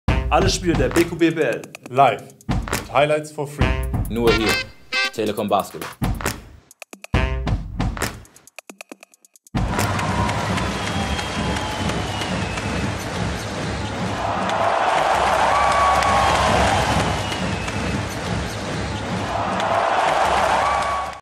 No-Look-Pass! K.C. Rivers zaubert im Fastbreak
Beko BBL-Debüt, und gleich mal Topscorer. K.C. Rivers zeigte sich beim Sieg des FC Bayern München über Oldenburg glänzend aufgelegt - wie sein No-Look-Pass im Fastbreak bestens zeigt.